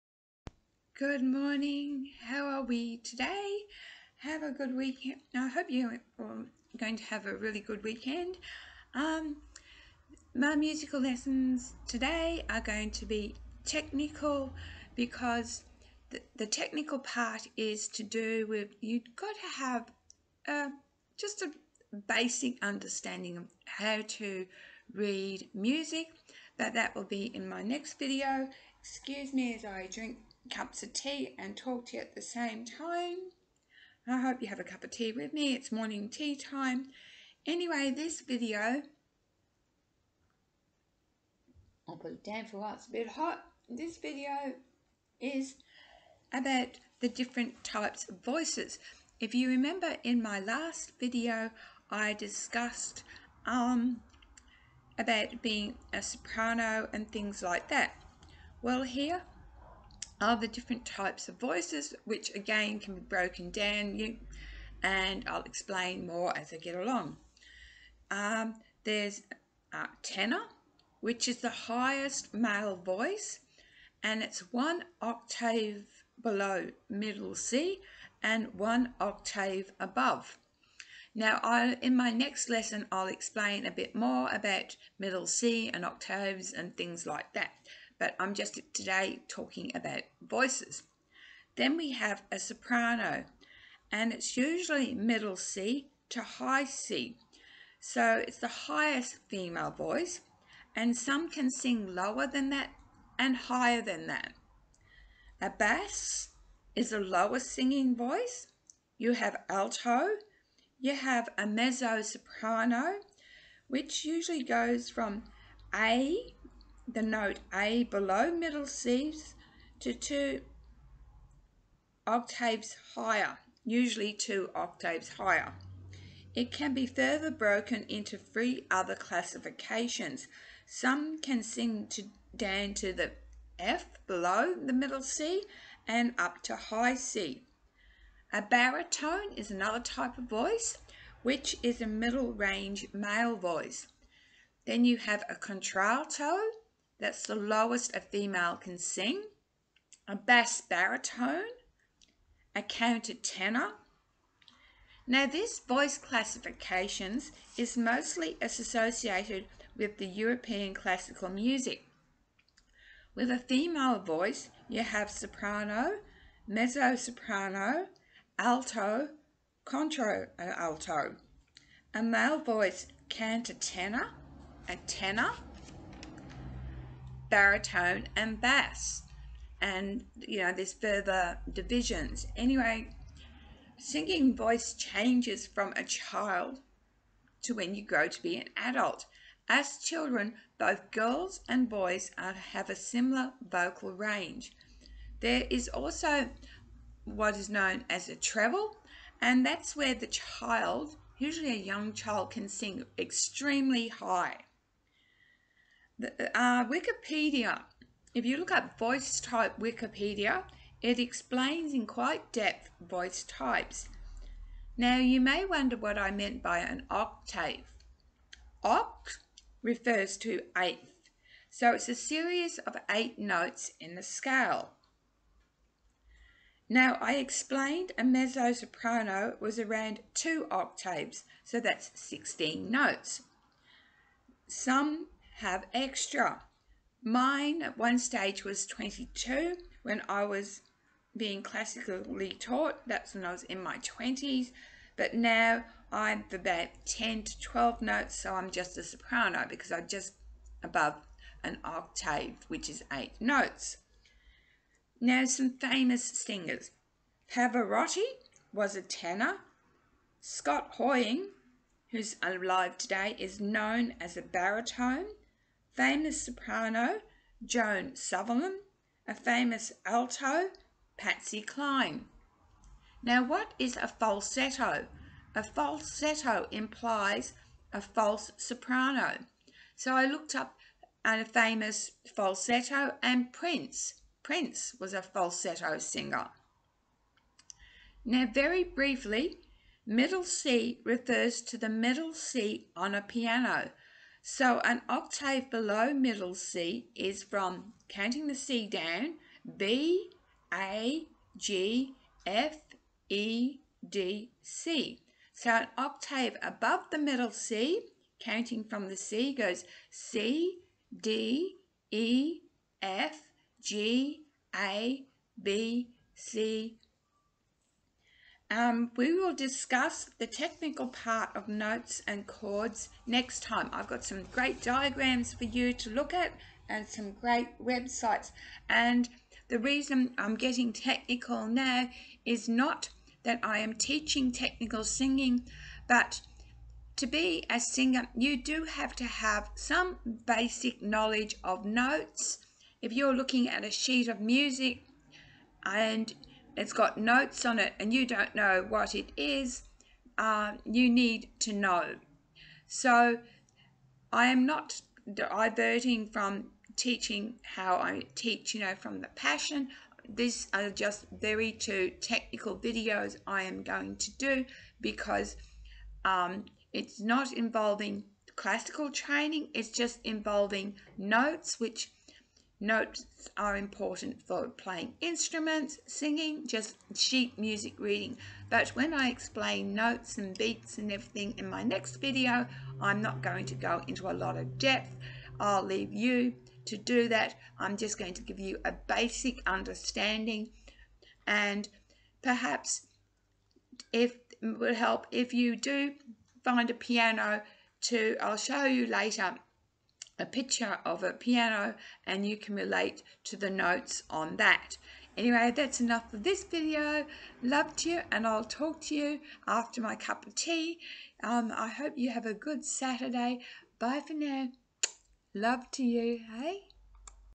singing lesson 2 types of singing voices descriptions
This is not actually a singing lesson as such but a description of types of singing voices such as tenor, soprano.  Very brief.